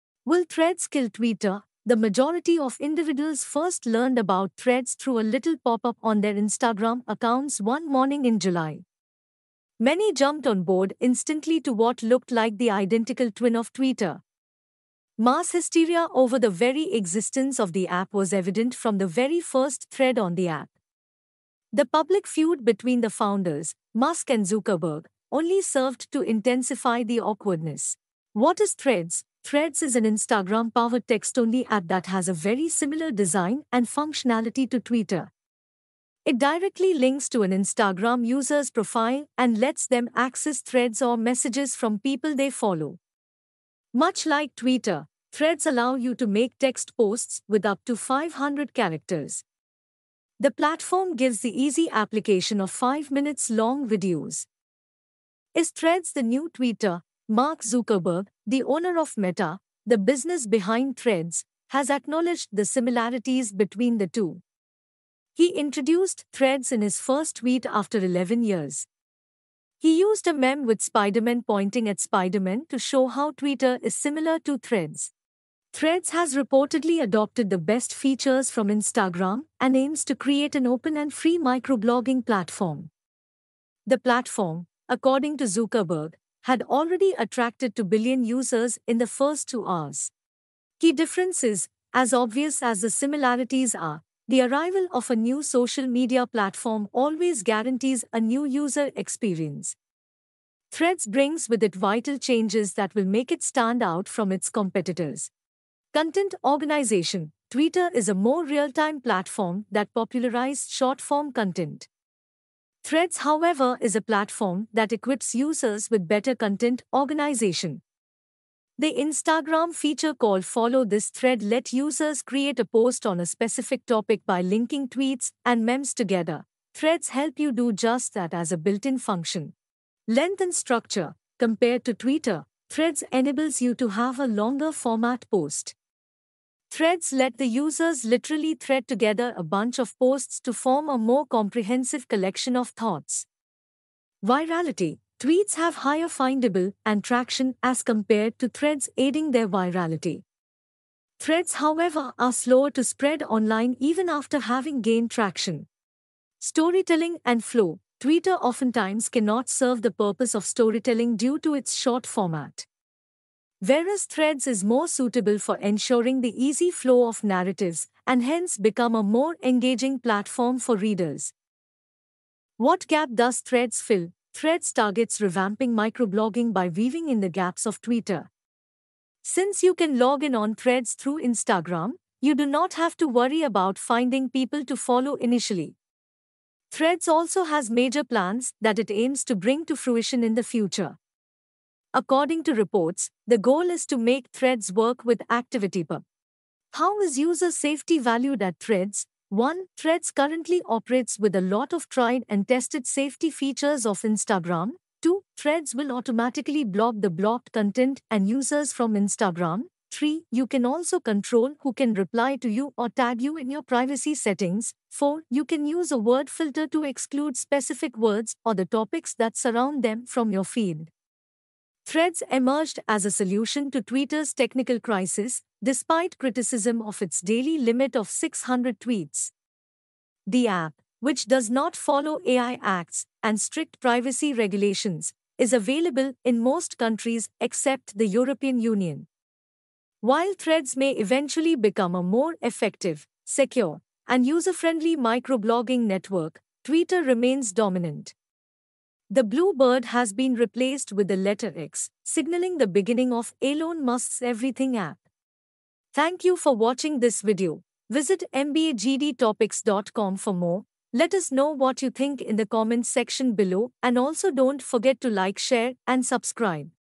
Will Threads kill Twitter? Know the key differences and similarities
What is Threads?
Threads is an Instagram-powered text-only app that has a very similar design and functionality to Twitter. It directly links to an Instagram user’s profile and lets them access ‘threads’ or messages from people they follow.
Much like Twitter, Threads allow you to make text posts with up to 500 characters. The platform gives the easy application of five minutes long videos.

Key differences:
As obvious as the similarities are, the arrival of a new social media platform always guarantees a new user experience. Threads brings with it vital changes that will make it stand out from its competitors.

1-Content organization:
Twitter is a more real-time platform that popularizes short-form content. Threads however is a platform that equips users with better content organization. The Instagram feature called “Follow this thread” let users create a post on a specific topic by linking tweets and memes together.
Threads help you do just that as a built-in function.

2-Length and Structure:
Compared to Twitter, Threads enables you to have a longer format post. Threads let the users literally thread together a bunch of posts to form a more comprehensive collection of thoughts.

3-Virality:
Tweets have higher findable and traction as compared to Threads aiding their virality. Threads however are slower to spread online even after having gained traction.
For more GD Topics and current affairs you can visit our